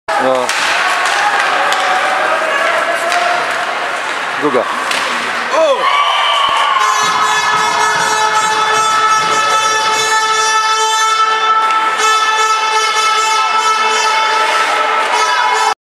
2 bramka Daniela Hołdowicza w meczu Żaków S na Bytomiu, 19 01 2013
2 bramka Daniela Hołdowicza w meczu Żaków Starszych, UKS MOSM Bytom - Tyskie Lwy MOSM Tychy, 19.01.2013